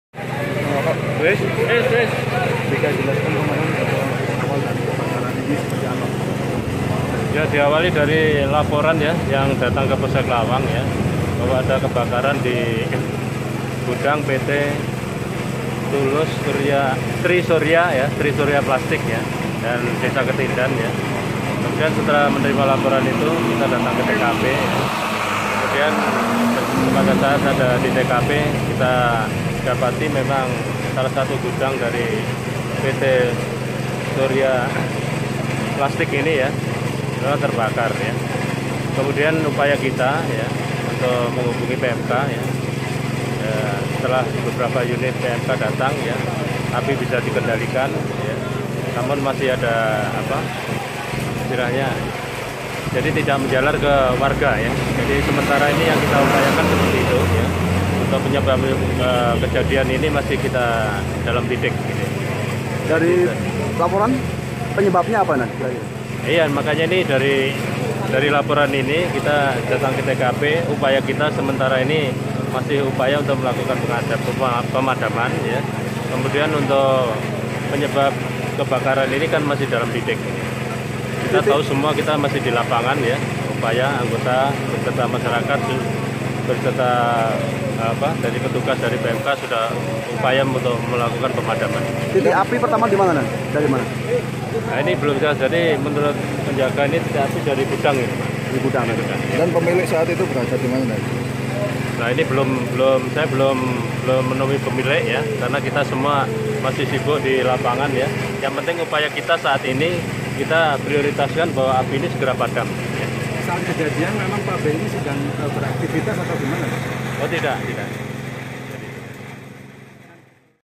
Breaking News- Pabrik Plastik Lawang Kembali Terbakar
MALANGTODAY.NET - Pabrik plastik PT Tri Surya yang beralamat di Jalan Ketindan, Lawang, Kabupaten Malang kembali mengalami kebakaran

Ini merupakan kali ke-empat Pabrik plasti PT. Tri Surya mengalami kebakaran,